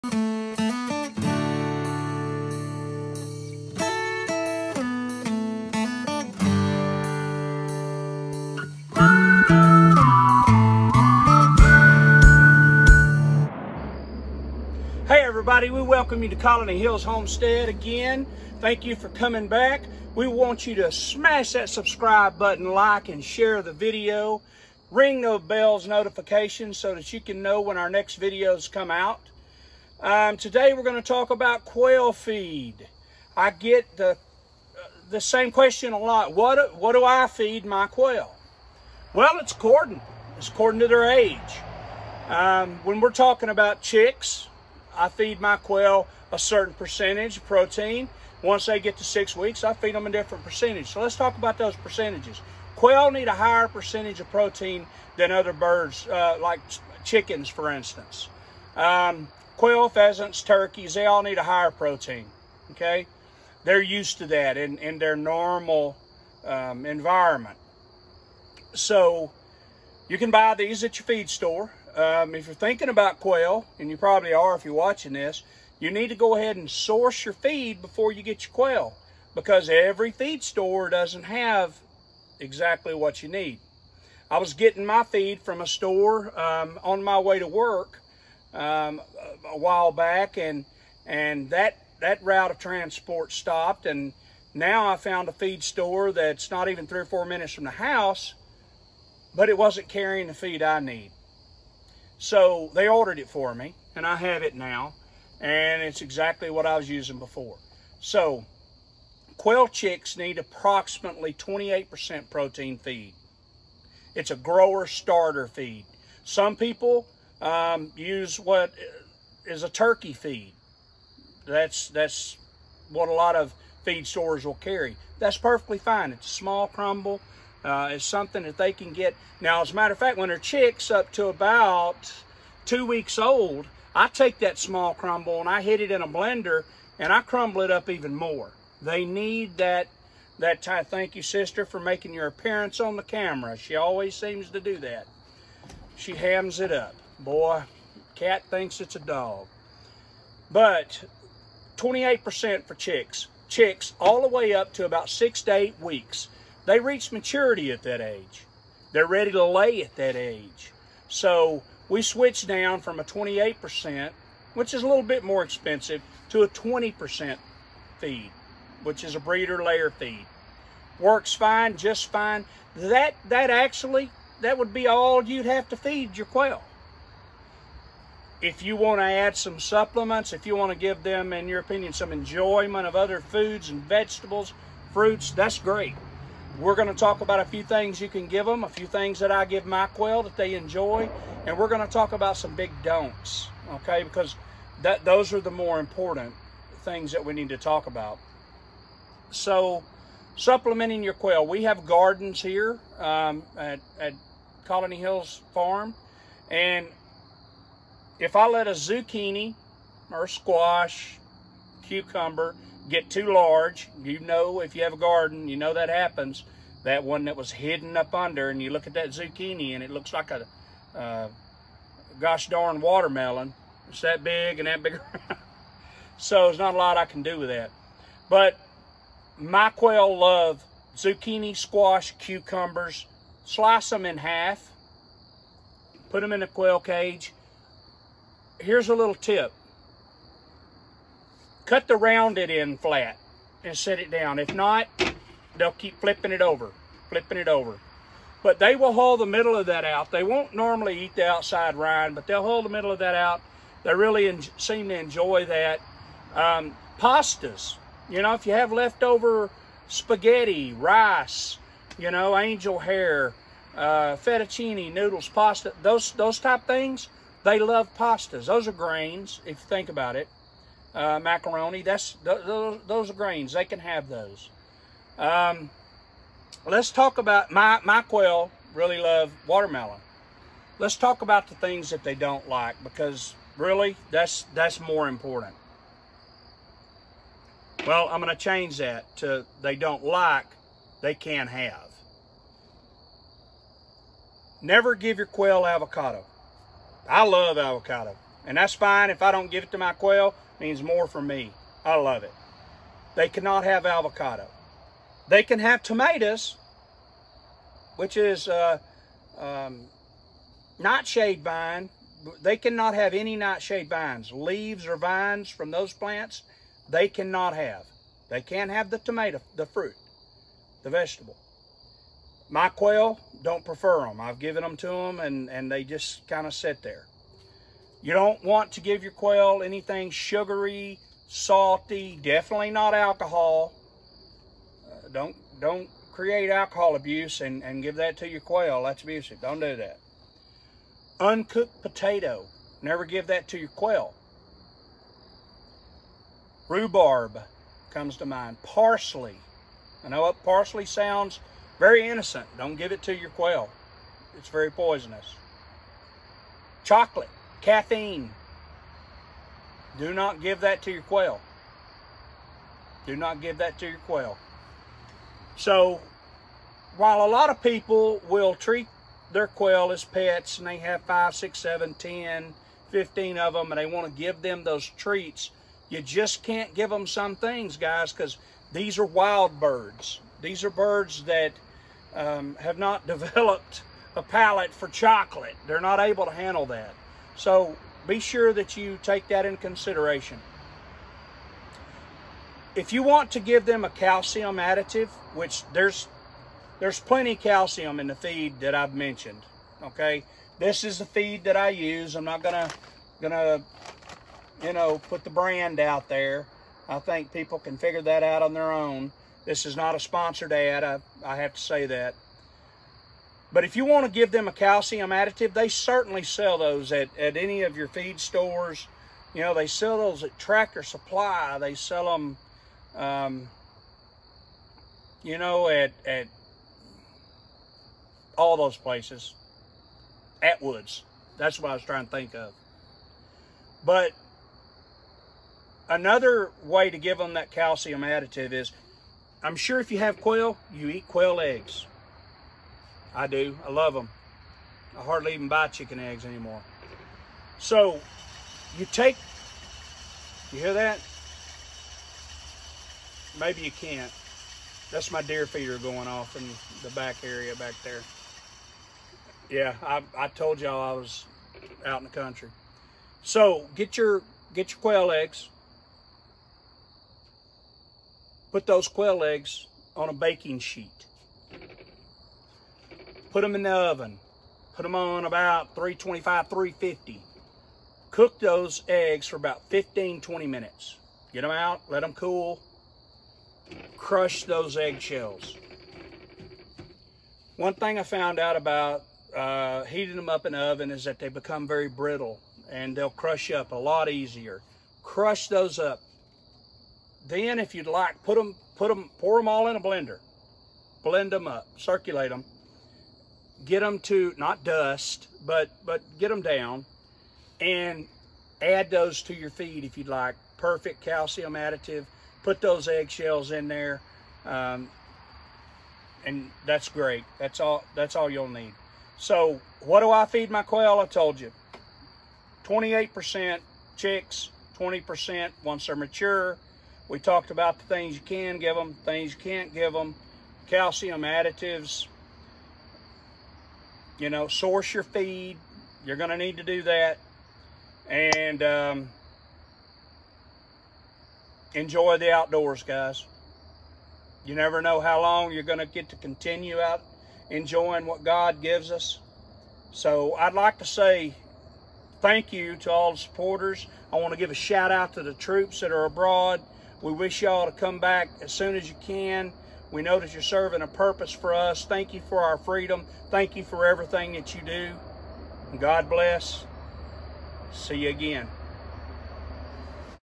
Quail Feeding-Do’s & Dont’s
Once you decide to raise quail, knowing how to feed them is a crucial piece of info! This video discusses where to find quail feed, protein needs, calcium supplementation, common foods that you can feed your quail, and those that you absolutely should avoid.

Looking for brooder supplies? Check the links below;
Poultry Waterer

Heat lamp

Pine Shavings
Interested in purchasing hatching eggs? Visit us on ebay!